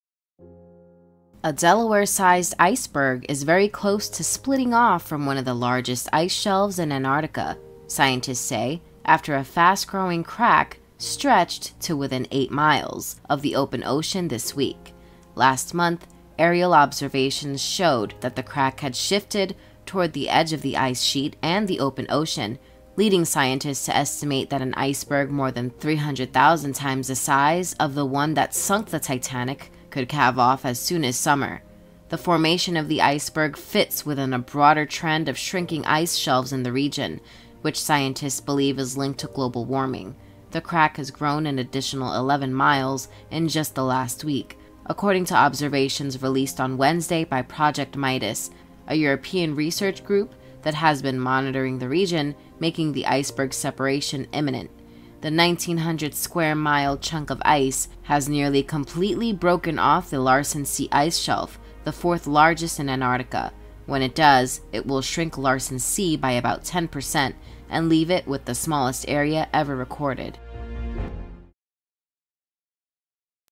Delaware Sized Iceberg Poised to Break Off Antarctic Shelf
A Delaware-sized iceberg is very close to splitting off from one of the largest ice shelves in Antarctica, scientists say, after a fast-growing crack stretched to within 8 miles (13 kilometers) of the open ocean this week. Last month, aerial observations showed that the crack had shifted toward the edge of the ice sheet and the open ocean, leading scientists to estimate that an iceberg more than 300,000 times the size of the one that sunk the Titanic could calve off as soon as summer. The formation of the iceberg fits within a broader trend of shrinking ice shelves in the region, which scientists believe is linked to global warming. The crack has grown an additional 11 miles (17 kilometers) in just the last week, according to observations released on Wednesday by Project MIDAS, a European research group that has been monitoring the region, making the iceberg's separation imminent. The 1,900-square-mile (5,000-square-kilometer) chunk of ice has nearly completely broken off the Larsen C ice shelf, the fourth largest in Antarctica. When it does, it will shrink Larsen C by about 10 percent and leave it with the smallest area ever recorded.